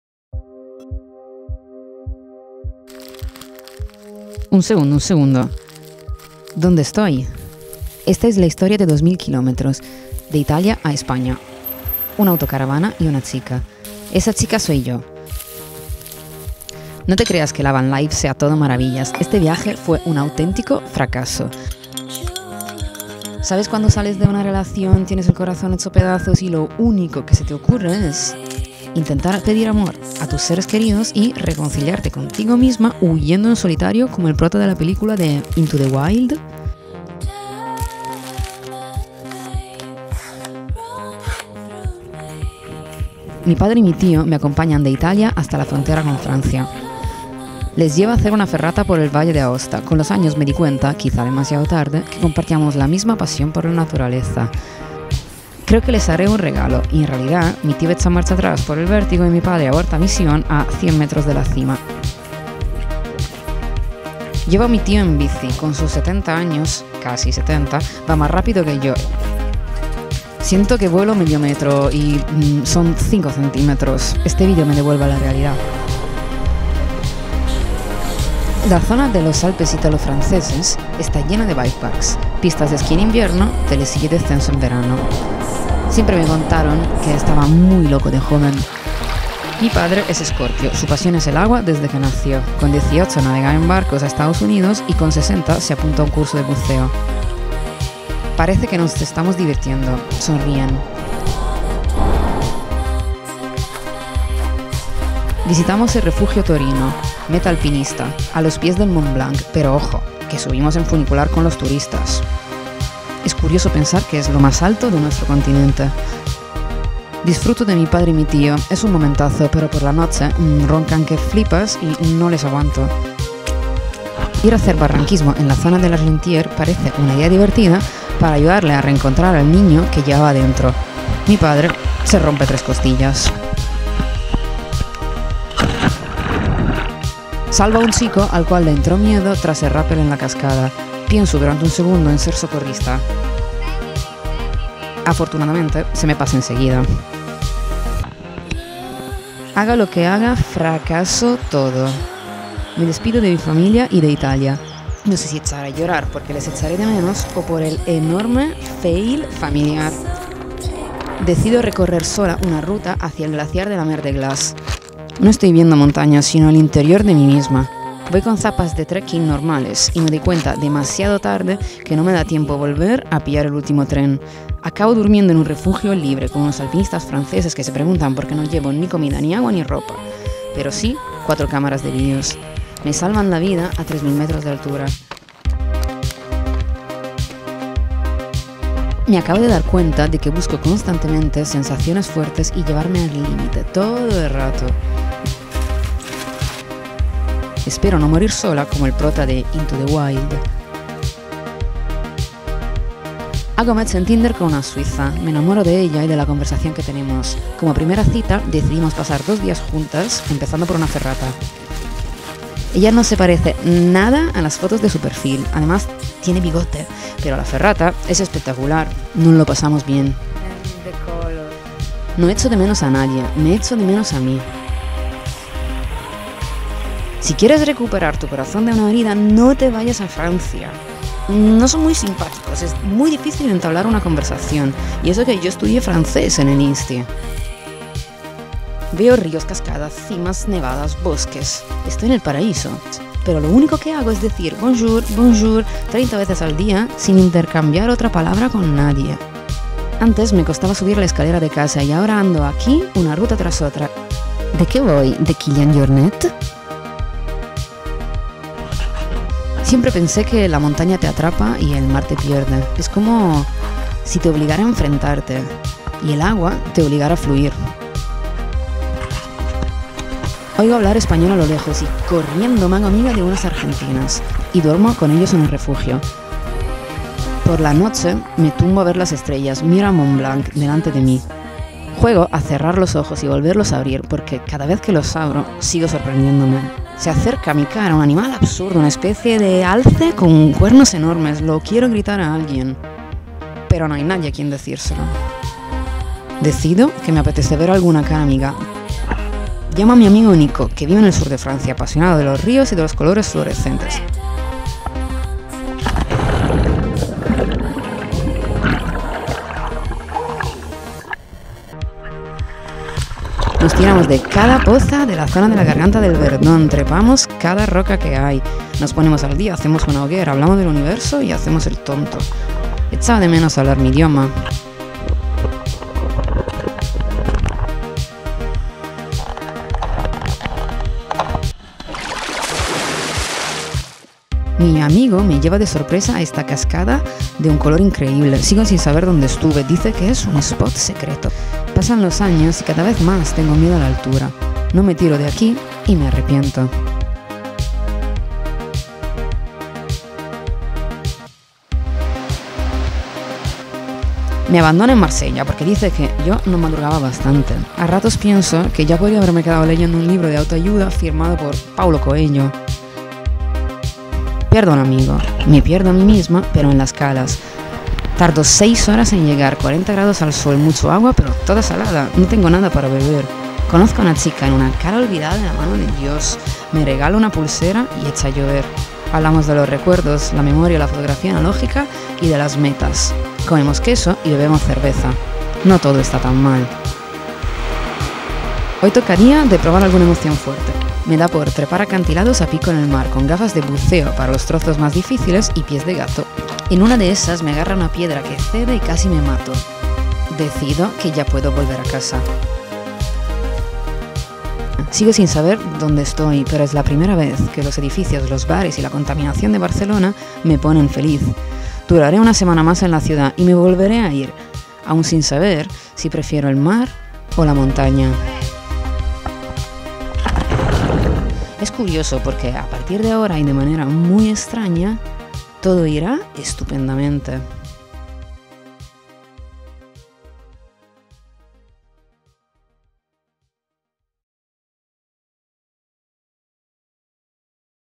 VIAJE en autocaravana: 2000 km en 7 minutos (de ITALIA a ESPAÑA pasando por FRANCIA)
Sigue mis aventuras en Instagram @Memyfriendsandthevan